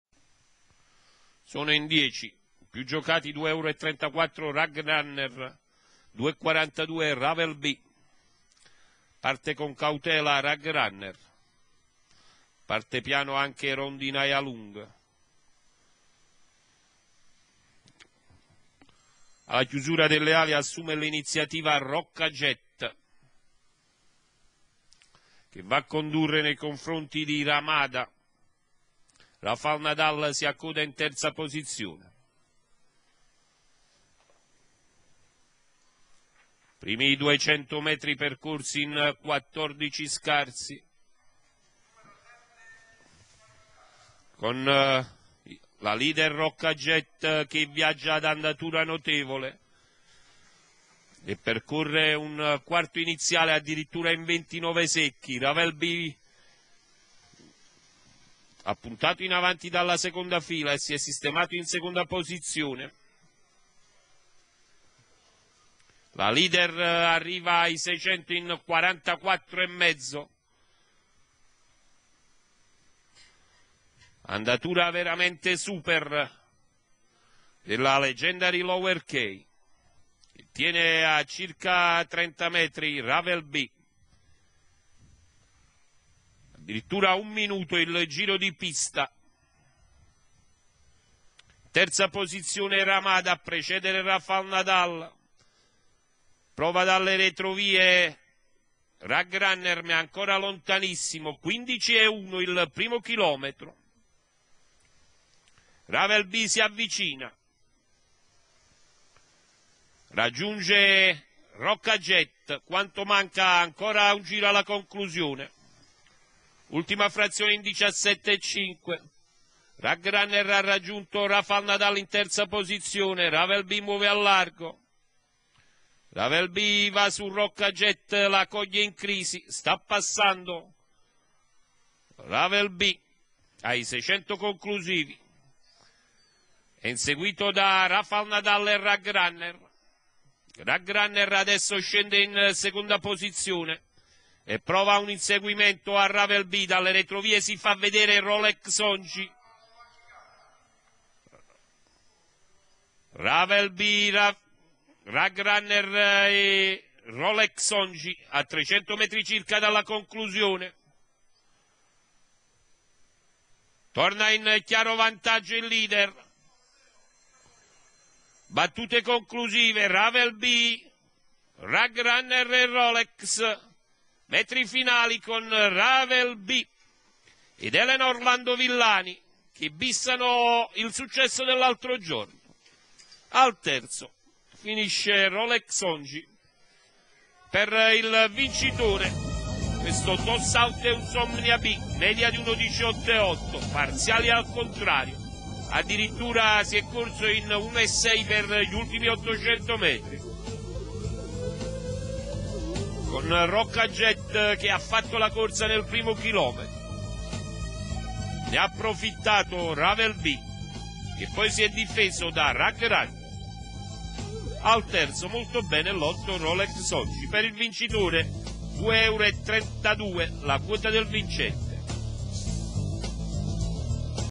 13/03/2013, Aversa Trotto, Premio Rose
1. RAVEL BI (VILLANI.OR. ELENA), 2. RAG RUNNER (BEVILACQUA B.), 3. ROLEX ONGI (FERRILLO P.), 4. RONDINAIA LUNG (ALDANESE GENNARO), 5. ROBY REGAL (D' ALESSANDRO VINCENZO), 6. RIGUADAGNO (SIMIOLI ANT.), 7. RAFAL NADAL (MINEO S), 8. RAMADA (BORRINO GIOVANNI), 99. ROCCA JET (LUONGO ANNA), 99. RACHELE CAF (SIMONE. GIOV. DE. JR)